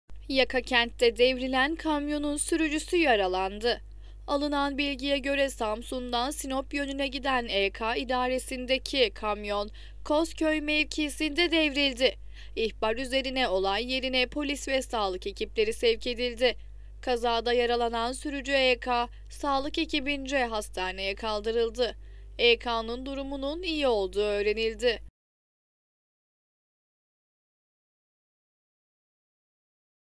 Samsun'da kamyon devrildi!
Samsun Haber
Samsun'un Yakakent ilçesinde devrilen 55 AIK 616 plakalı kamyonun sürücüsü yaralandı.